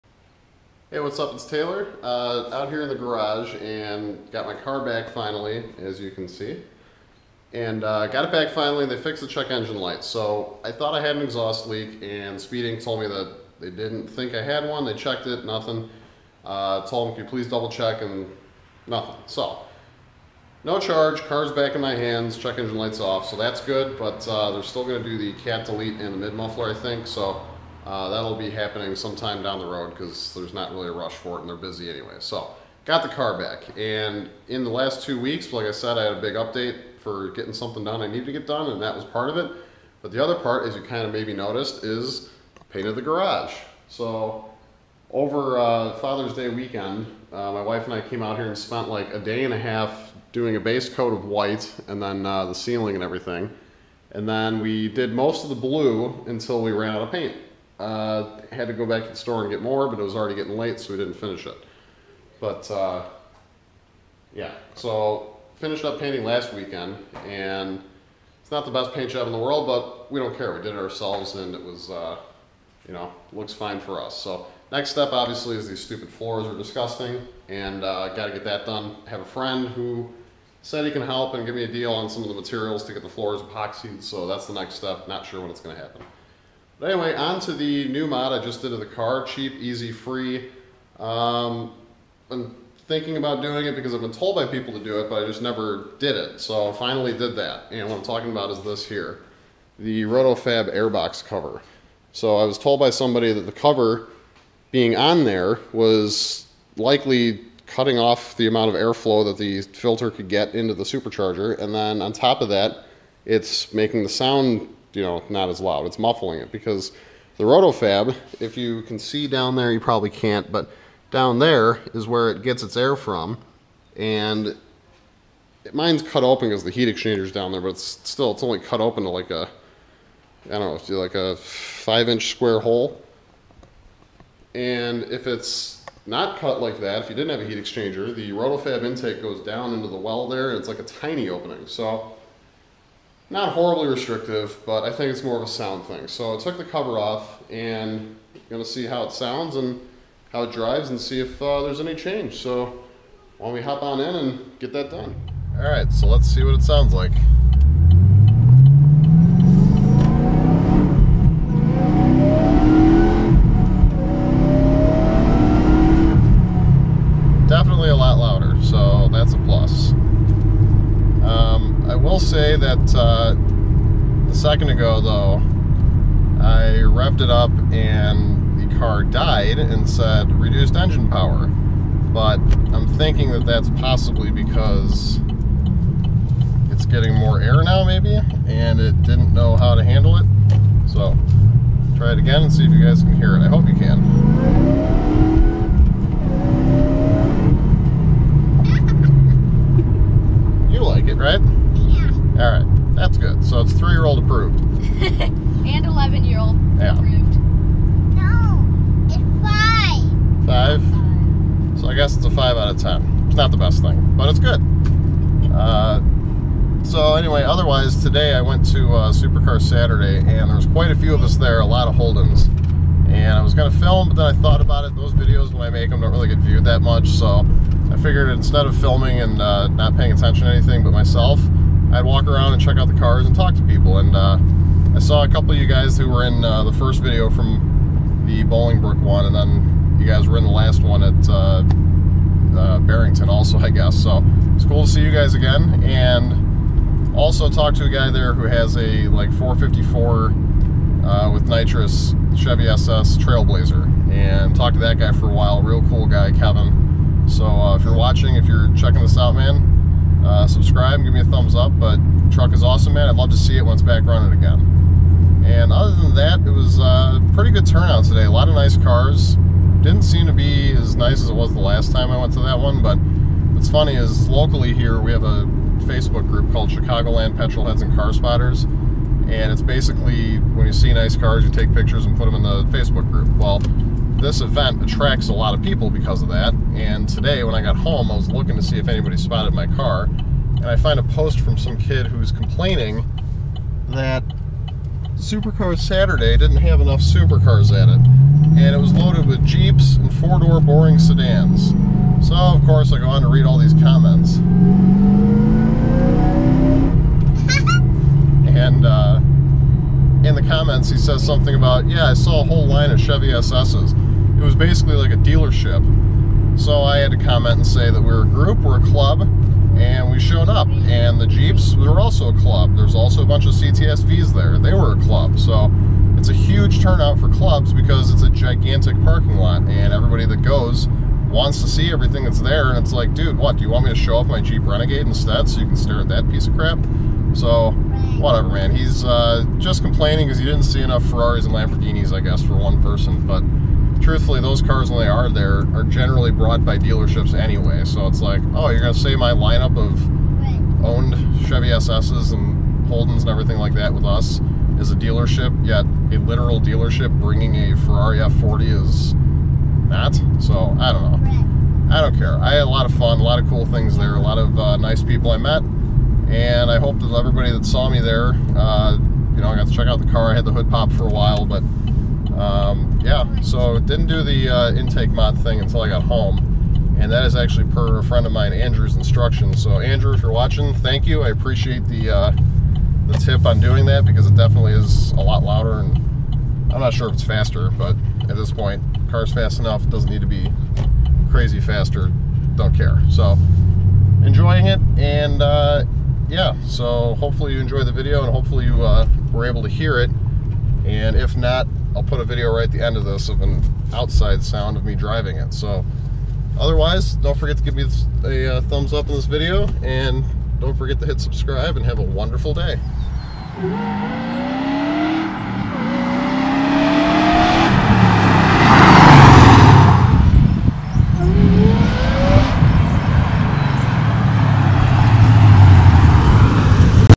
Mod I should have done MUCH sooner!
Modified something on the car myself that I wish I would have done a long time ago. Really easy and definitely worth it. I also went to Supercar Saturday again today but apparently some people weren't happy that there was a huge turnout of Holden cars!